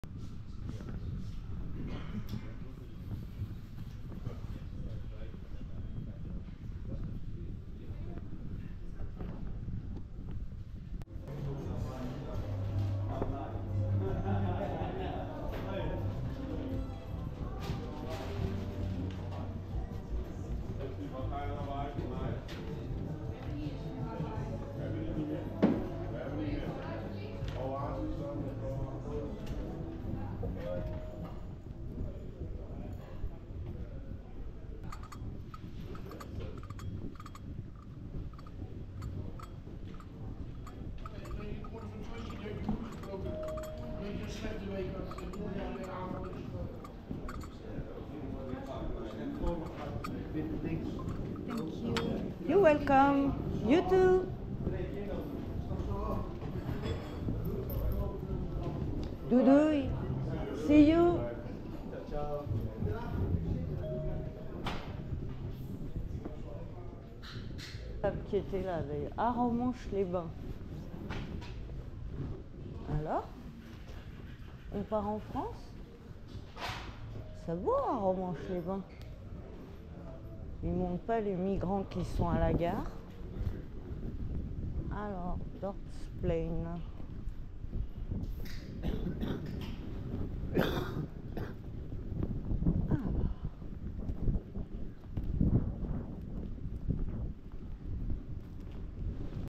ZANDVOORT 🇳🇱 The best coffeeshop is ... the YANKS !
S'il vous faut encore un dernier argument pour venir à Zandvoort ... vous y trouverez le meilleur coffeshop de toute la région : le Yanks, situé sur une place tranquille (Dorpsplein), ouvert de 9h à 3h du matin, avec une terrasse ensoleillée, deux étages très confortables (avec des billards au 1er), un menu exhaustif ... et surtout une ambiance détendue sans touristes (sauf l'été et le week-end du grand prix bien sûr). Que demander de plus ? (1/03/2024, film n°9)